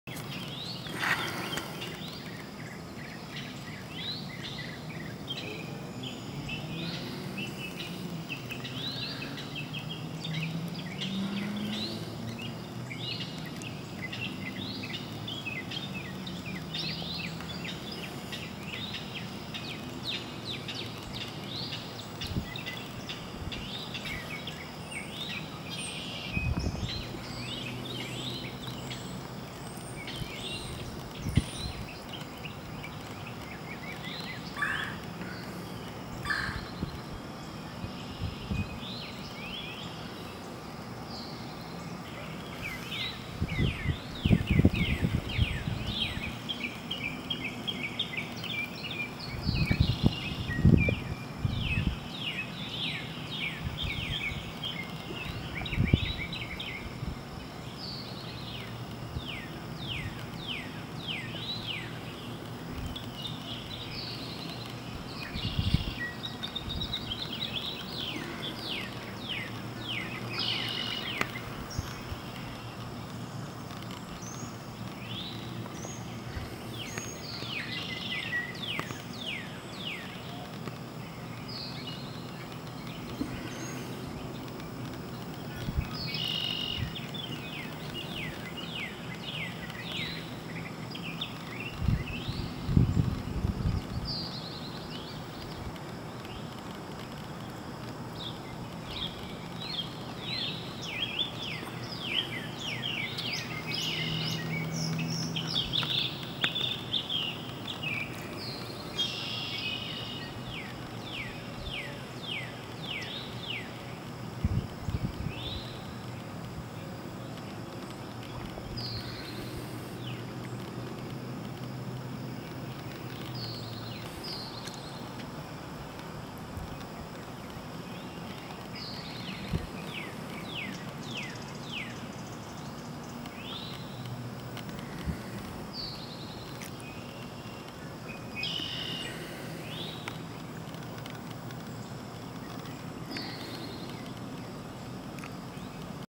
2012March22 Bayou Birds - Mary Walker Bayou - Mississippi
These 3 Birds came up behind my house on March 22, 2012. Took some Video because I have never seen this species around my yard before. Maybe someone can tell me what kind they are and provide a link with some more information. Interesting Creatures.